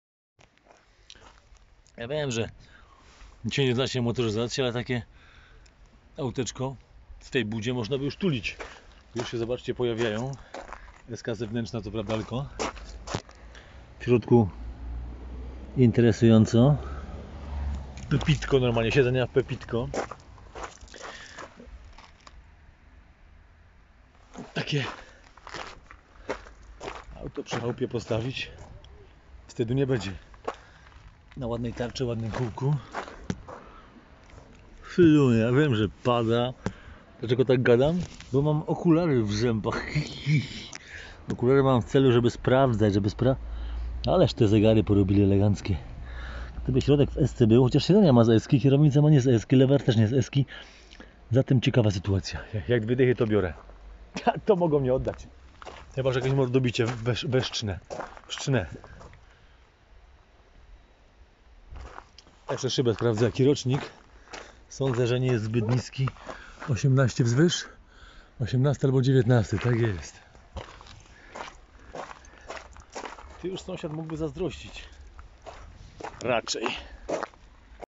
Audi a6 2019 rok s-line...dopiero co zjechała z lawety na plac
kolejny wyjątkowo zacny pojazd na placy z Autami..